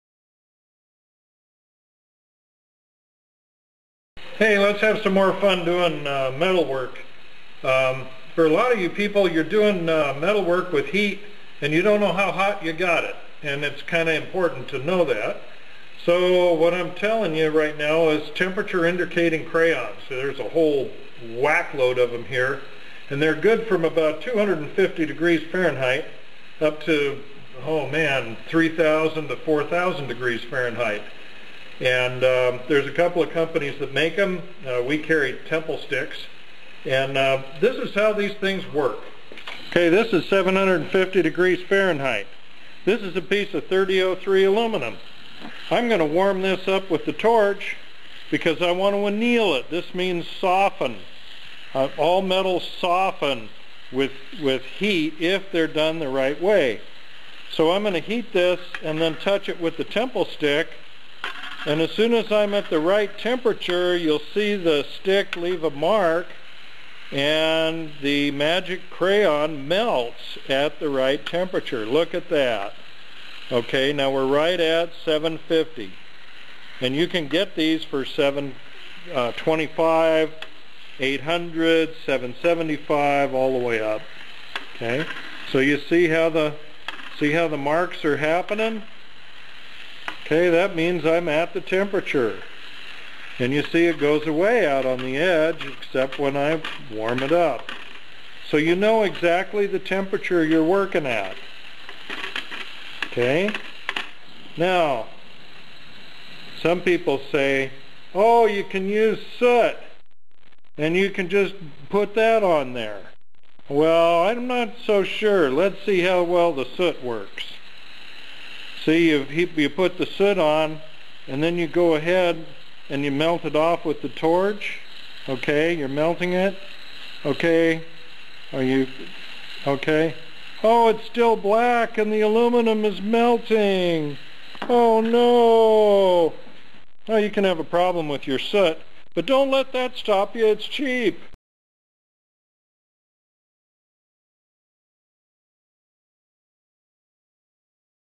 Temperature Crayons - Metalworking Tips from TM Tech
Kent White demonstrates using Tempilstiks to test the temperature of metal for hot working.